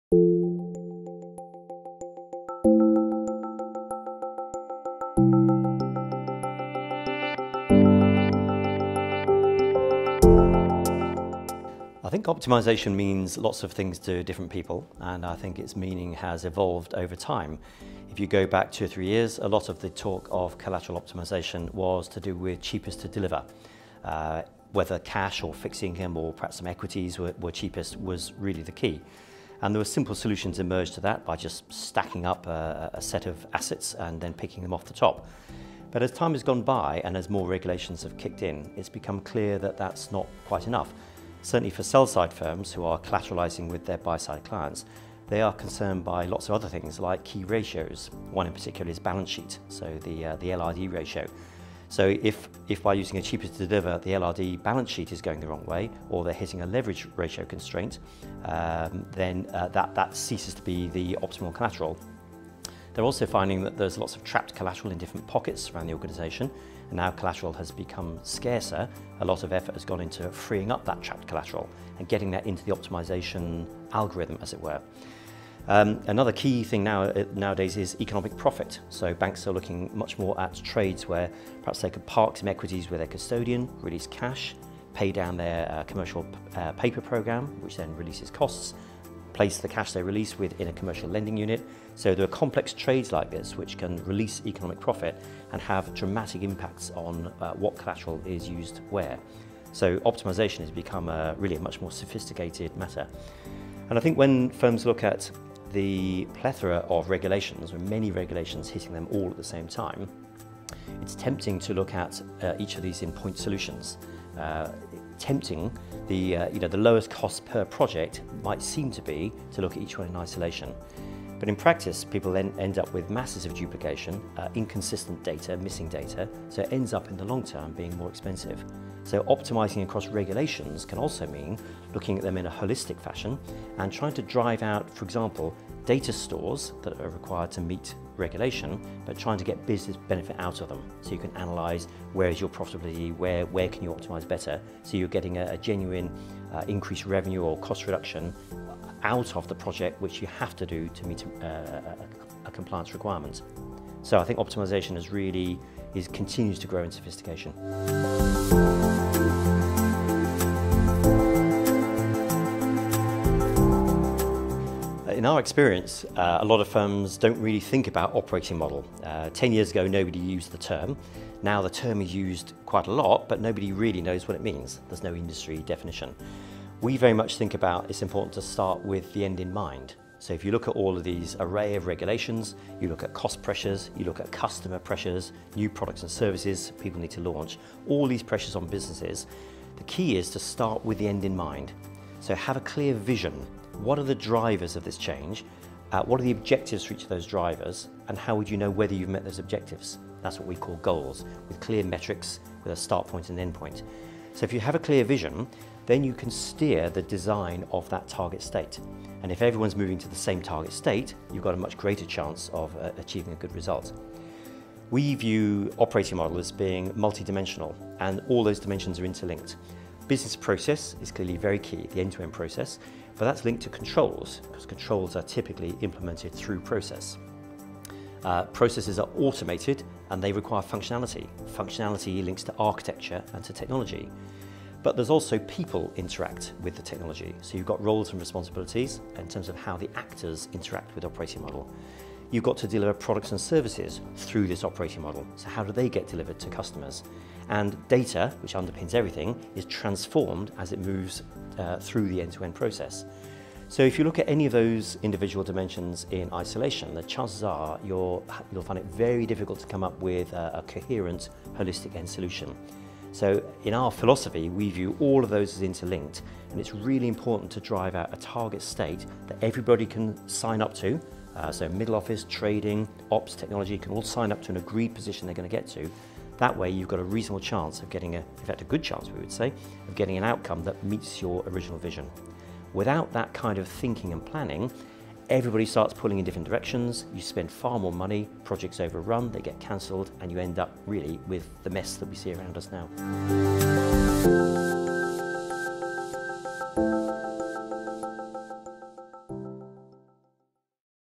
Trends and challenges in collateral management
David Field, Managing Director, The Field Effect, explains why taking a
strategic approach to collateral management is necessary to achieve long-term
competitive collateral optimization and manage the trend towards constant regulatory changes that we have seen over the last 10 years. A trend that is likely to continue.